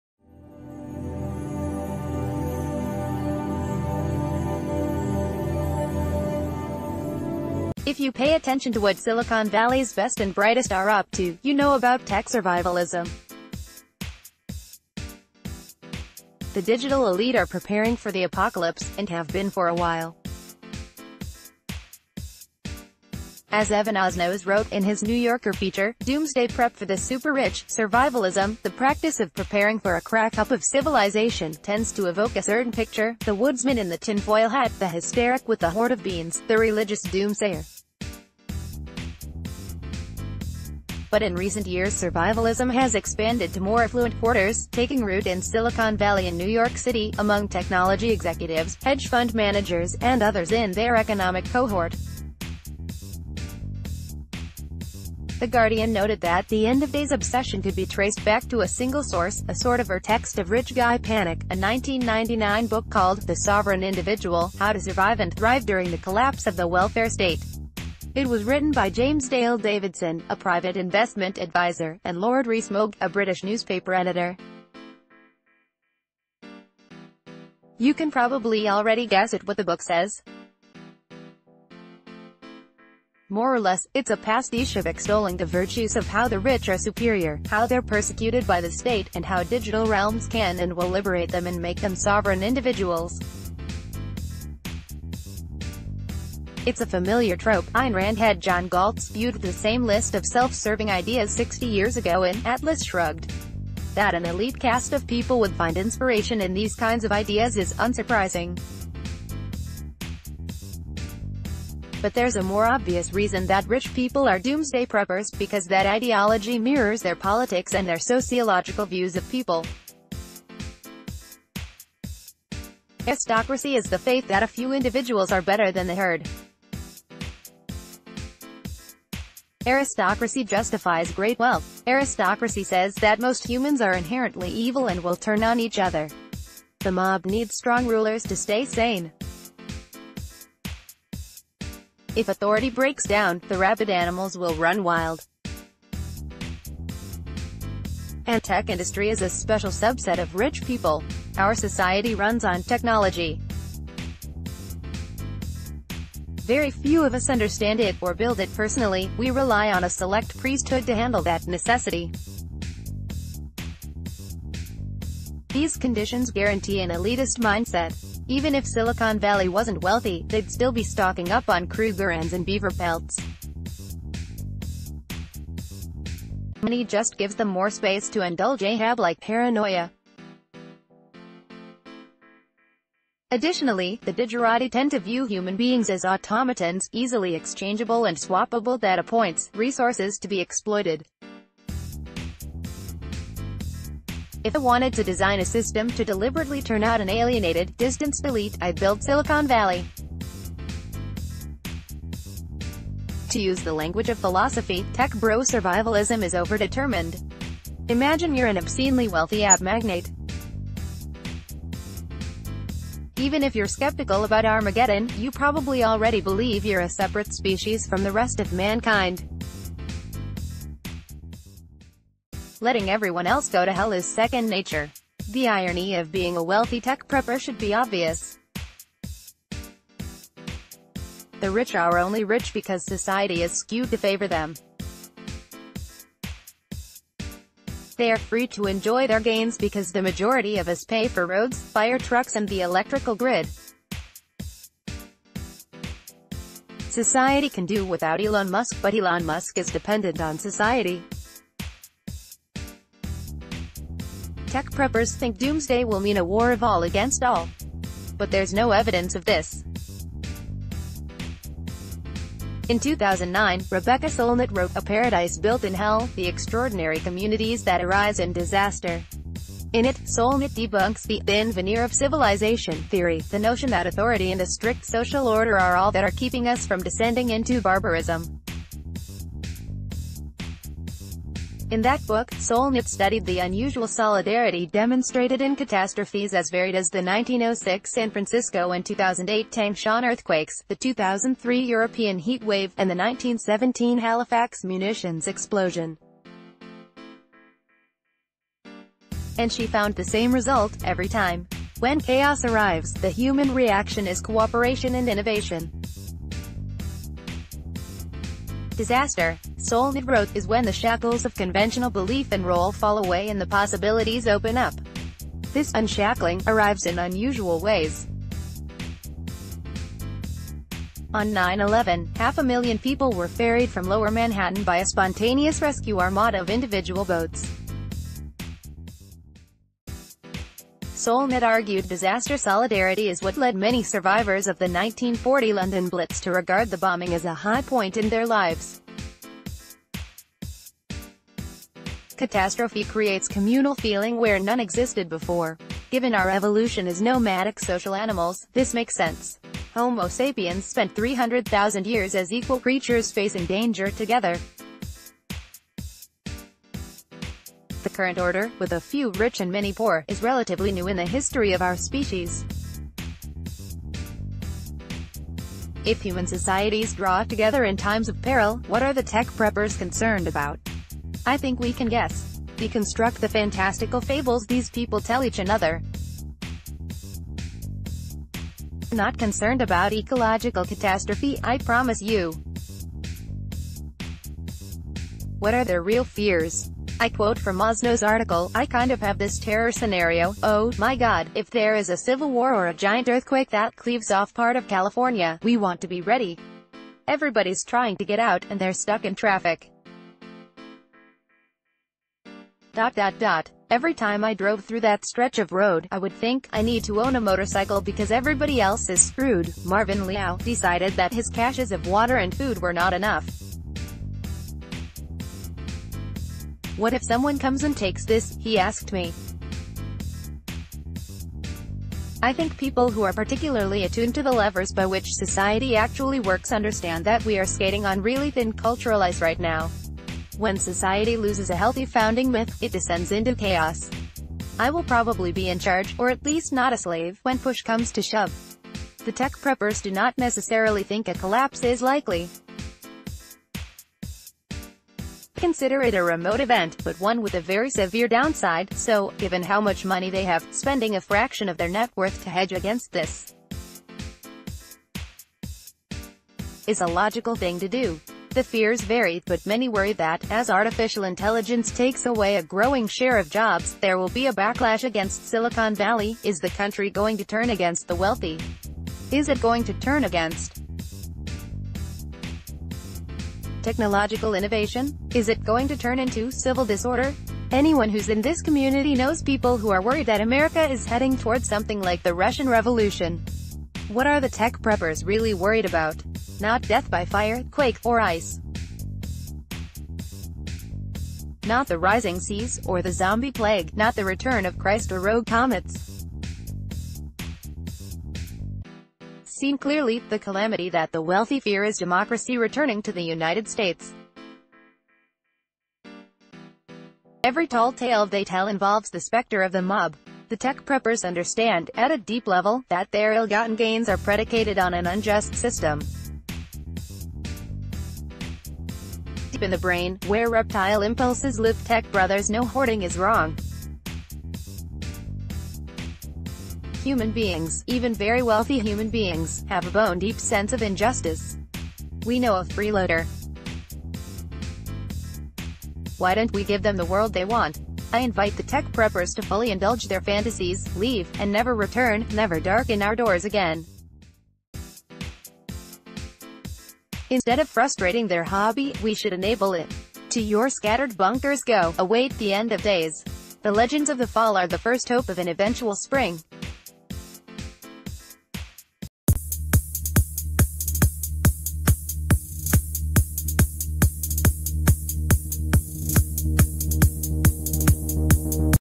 Breaking News | Here is the real reason why tech billionaires are prepping for doomsday
If you pay attention to what Silicon Valley’s best and brightest are up to, you know about tech survivalism. The digital elite are preparing for the Apocalypse, and have been for a while. As Evan Osnos wrote in his New Yorker feature, “Doomsday Prep for the Super-Rich,” Survivalism, the practice of preparing for a crackup of civilization, tends to evoke a certain picture: the woodsman in the tinfoil hat, the hysteric with the hoard of beans, the religious doomsayer. But in recent years survivali...